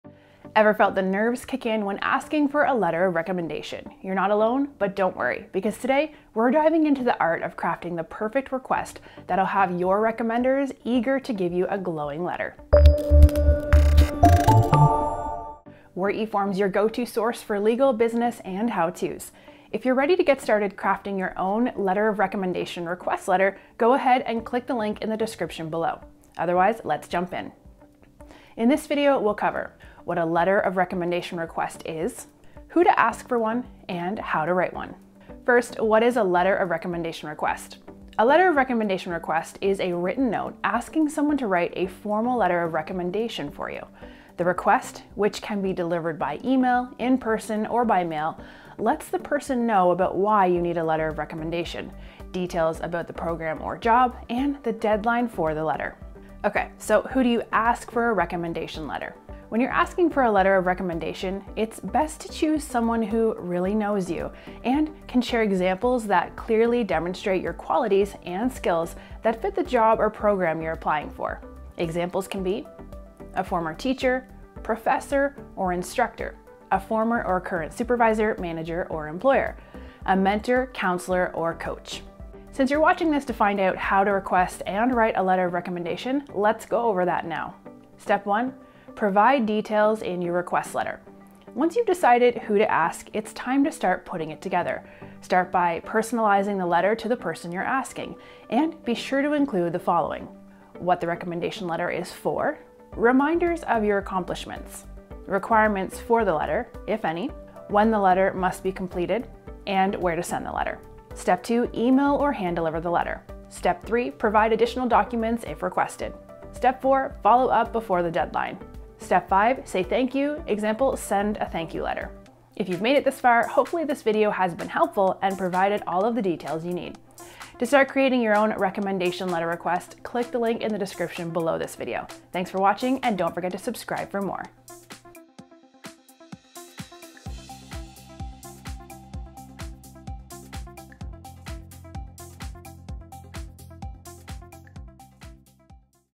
Crafting the Perfect Letter of Recommendation Request
A letter of recommendation request is a written note asking someone to write a formal letter of recommendation for you. In this video, we break down our top tips for crafting the perfect request that will have recommenders eager to sing your praises.

0:00 Intro
0:36 What is a letter of recommendation request?
0:58 Who do you ask for a Recommendation Letter?
1:28 Steps for writing the letter
2:03 Conclusion and where to get a template